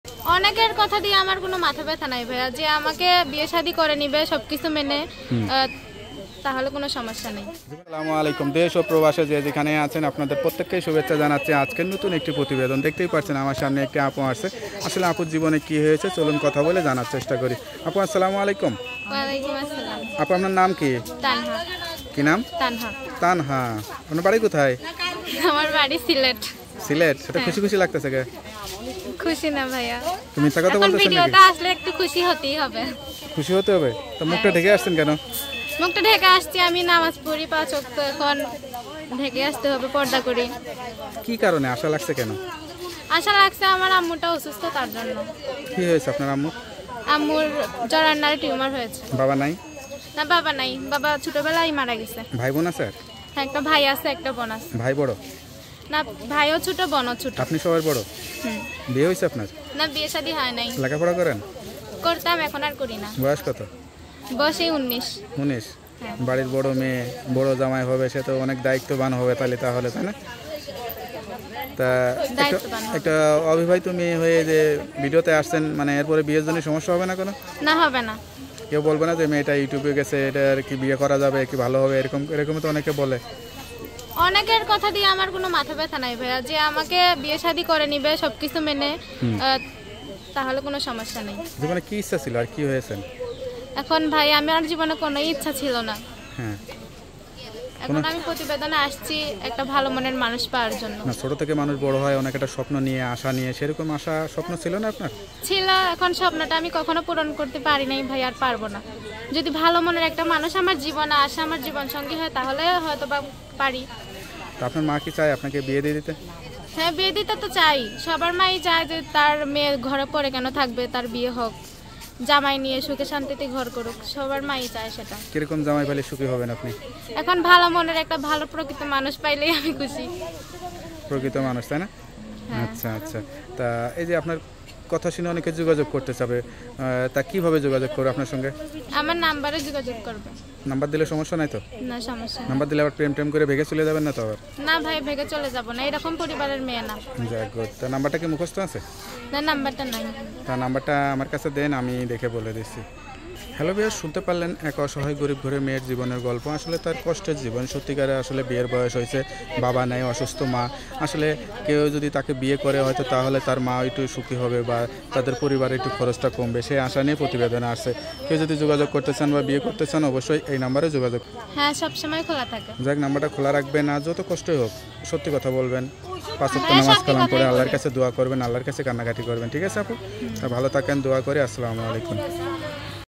খুবই গরীব ও অসহায় ঘরের একটি মেয়ের জীবনের কতটা অসহায় হলে প্রতিবেদনে আসতে হয় জানতে দেখুন।
খুবই গরীব ও অসহায় ঘরের একটি মেয়ের জীবনের কতটা অসহায় হলে প্রতিবেদনে আসতে হয় জানতে দেখুন। কেউ যদি তার সঙ্গে যোগাযোগ করতে চান তাহলে তার মোবাইল নাম্বারটা ভিডিওর মধ্যে দেওয়া আছে ওই নাম্বারে যোগাযোগ করুন ধন্যবাদ।